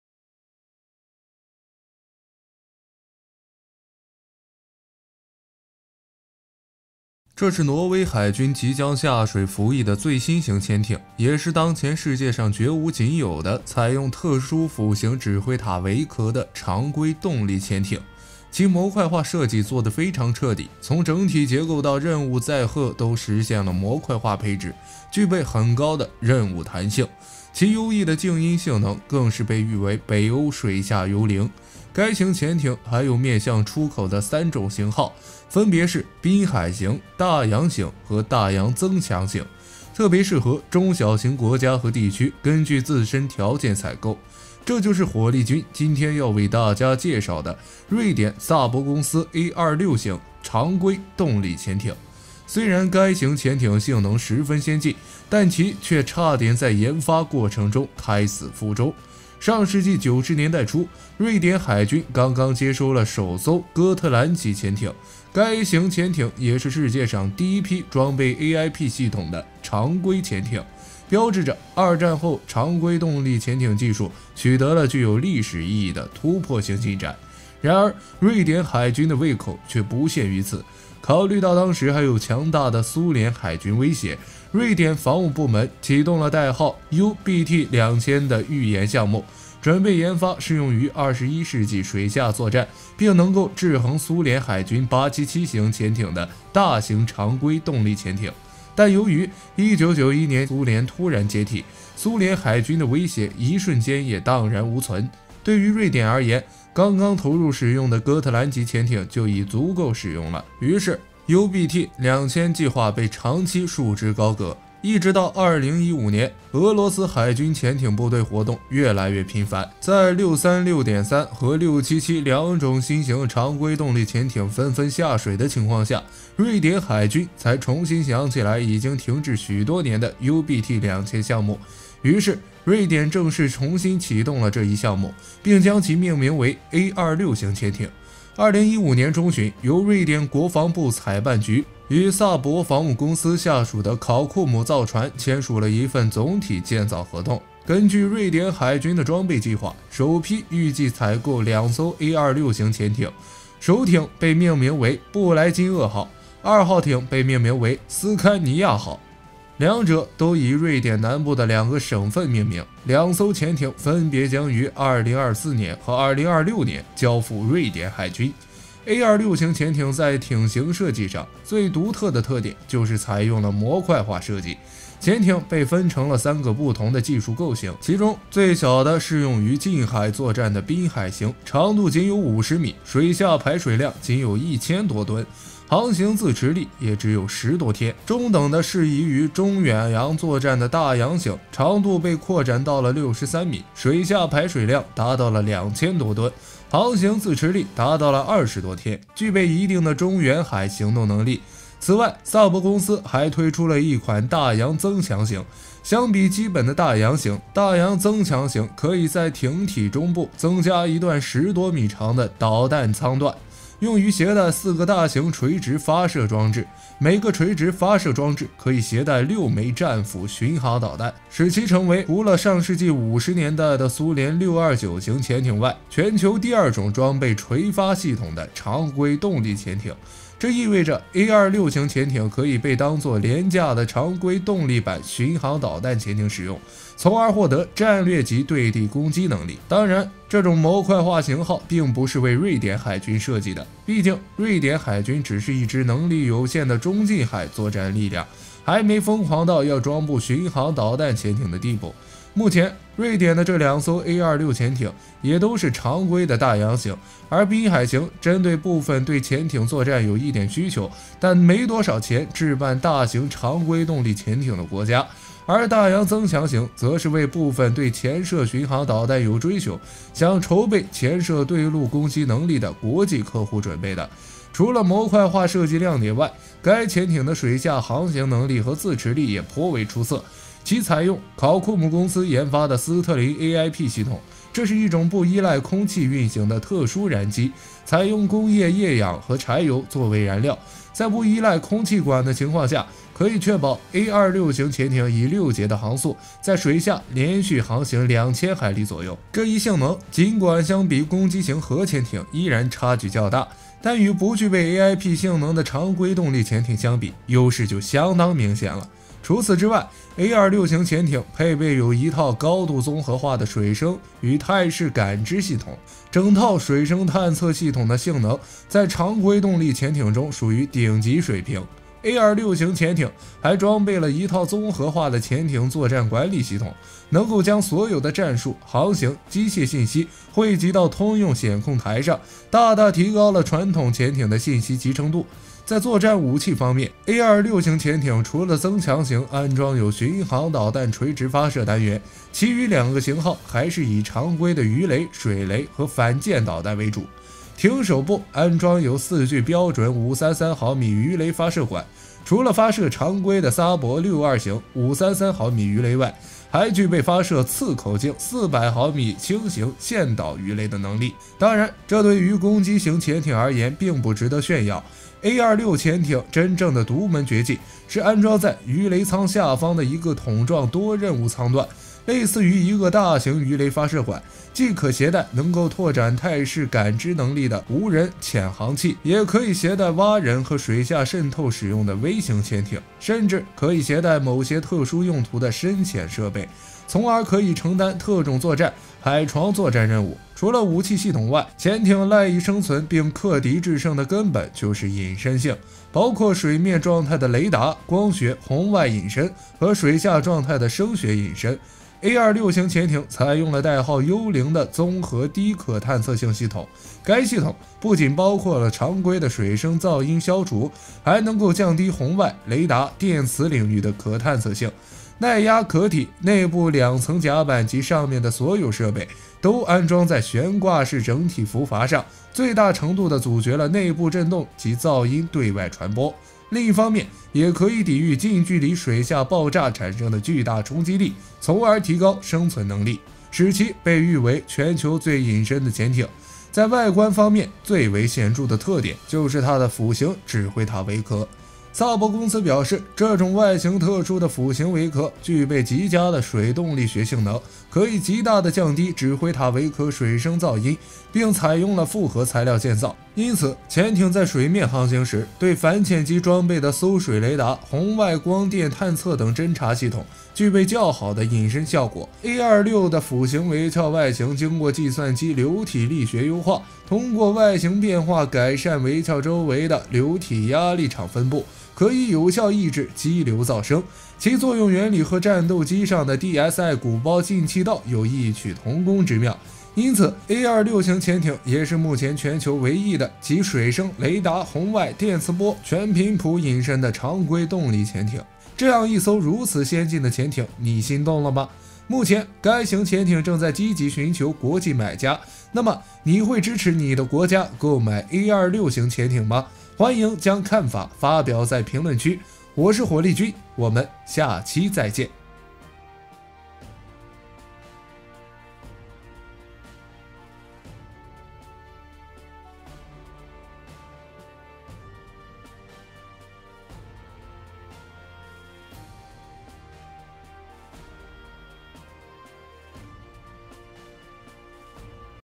瑞典A26型潛艇，全球最強全頻譜隱身常規動力潛艇，被譽為北歐幽靈
布萊金厄級潛艦是瑞典紳寶考庫姆公司所設計開發的一款常規動力潛艇，開發計畫最初稱為「U-båt 2000」，後改名A26型（英語：A26），瑞典國防軍所屬之瑞典國防裝備管理局（英语：Defence Materiel Administration）則稱其為NGU（Nästa generations ubåt，次世代潛艦）。
由於2014年德國蒂森克虜伯集團收購考庫姆公司，開發計畫曾一度遭德國入主的經營團隊給阻擾，德國方的經營團隊要求開發成本必須全部由使用者承擔，而非是當時的分攤開發成本模式。這些經營爭議導致2014年4月8日瑞典政府動用武力強制收繳該公司中屬於瑞典國防裝備管理局的所有設計資料及設備。公司的經營紛擾先是由紳寶集團與考庫姆公司簽訂收購意向書，2015年3月18日瑞典政府決定以82億瑞典克朗的價格訂購2艘A26潛艦，此後紳寶集團出資收購考庫姆公司後才告落幕。目前兩艘布萊金厄級皆由卡爾斯克魯納造船廠（瑞典语：Karlskronavarvet）承造，預定在2024年下水。